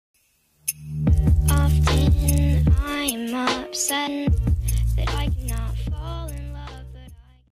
New Outro | Free Download/Record
You may have this outro I edited a few more parts, I don’t have a download link so just screen record it to have it,